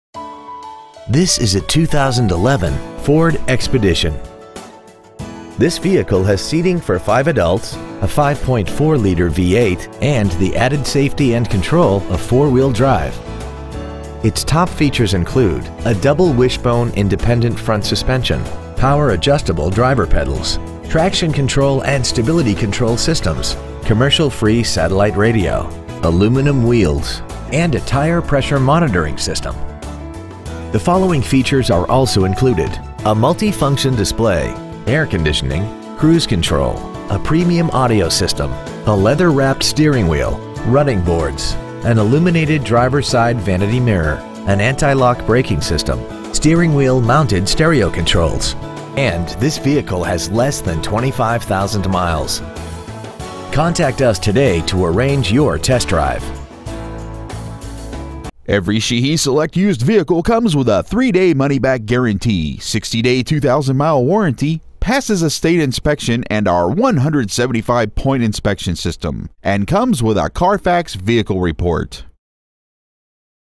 2011 Ford Expedition Warrenton VA 20187
Year : 2011
 Make : Ford
 Model : Expedition
 Engine : Gas/Ethanol V8 5.4L/330
 Trans . : Automatic
 Exterior : Black
 Miles : 24,209

 Stock : YP0556